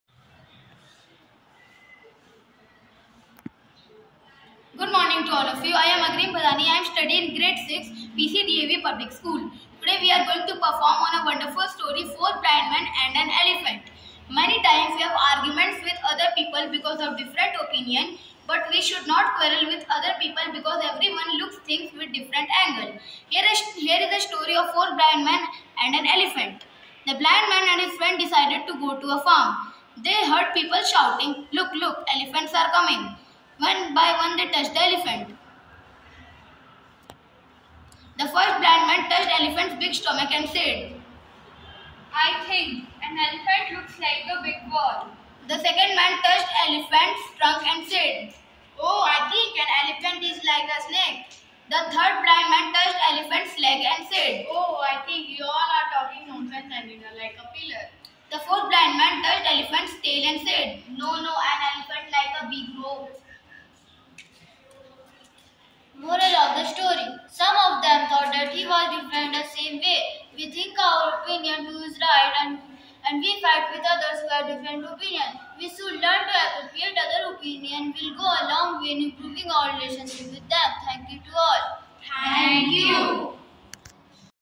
"Story telling" Enhances communication skills. l PC DAV Public School
*"Story telling" Enhances communication skills.It is the skills help learners to structure ideas, organize information, and communicate effectively. By narrating stories, learners can improve their speaking, reading, and writing skills.*